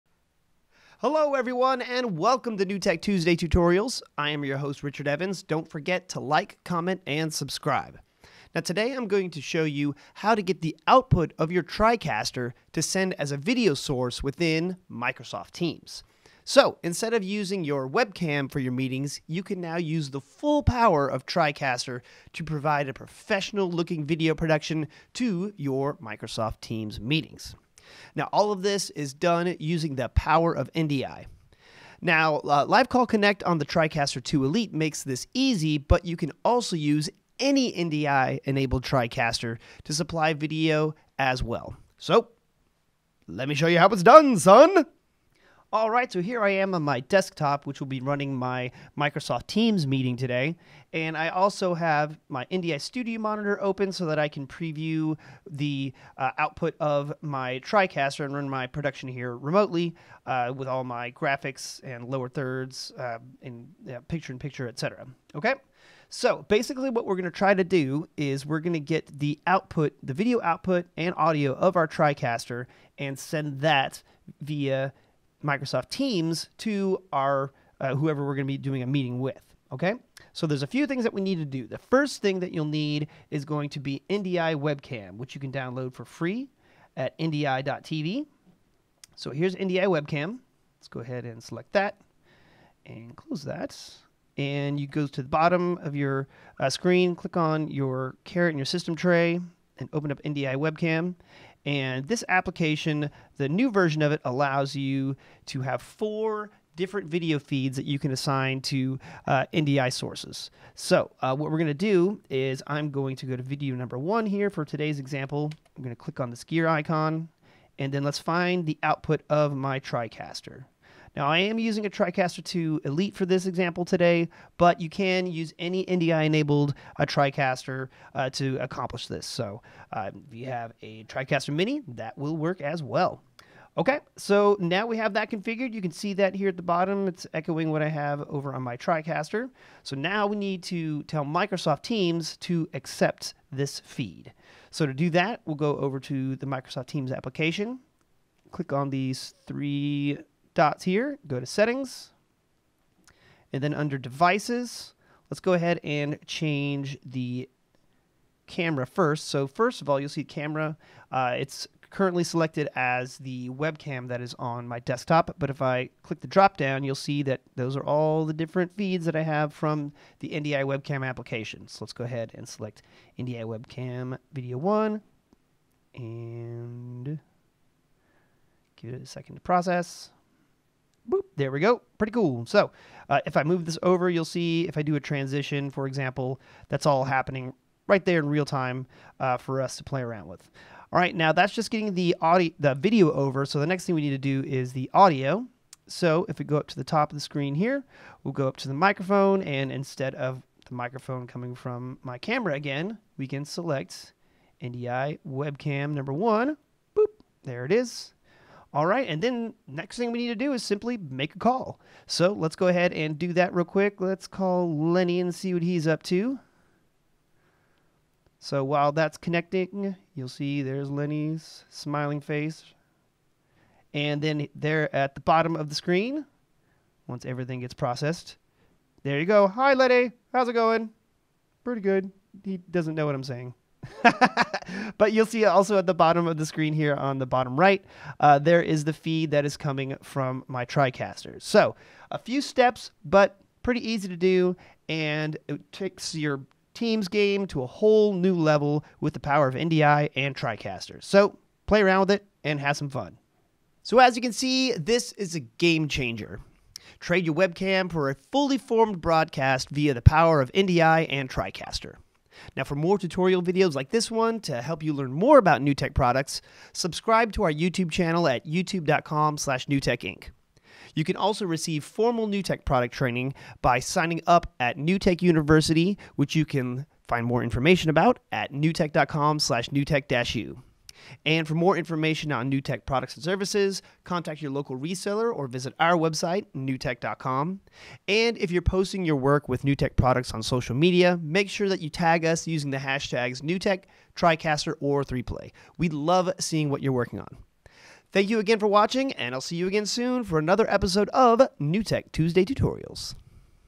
Tuesday Tutorials : Using TriCaster with Microsoft Teams
With NDI Webcam and TriCaster you can supply a professional broadcast to your Microsoft Teams meetings.